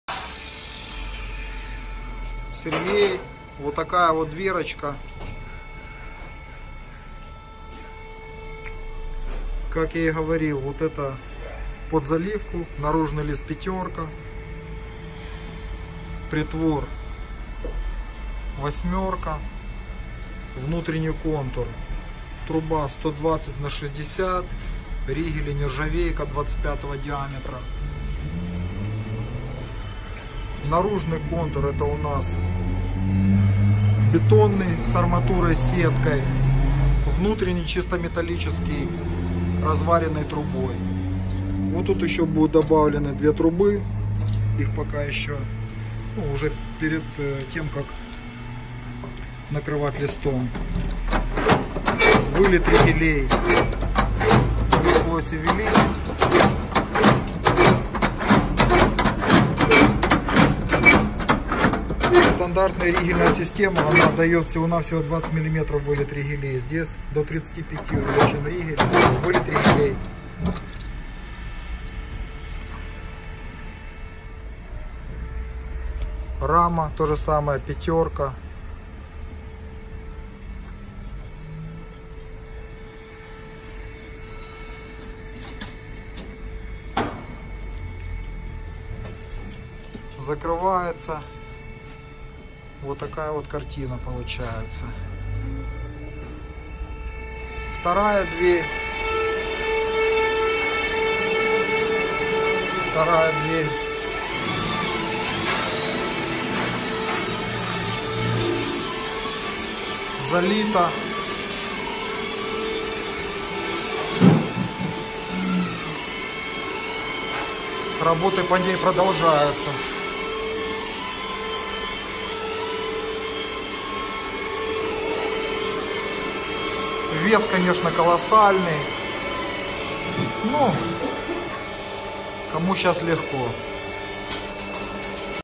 Бункерные двери СТЕНА. Антирейдерские, сейфовые двери от Монолит
Антирейдерские двери СТЕНА.
Оказывают достойное сопротивление тяжелому электро инструменту, газовому резаку, отбойному молотку. Весят больше тонны, имеют железобетонную конструкцию и толщину от 18 сантиметров.  Выдерживают автомат Калашникова, пулемет, взрыв гранаты.

Для заказа обращайтесь по телефону:
г. Киев, ул. Вискозная, 3Б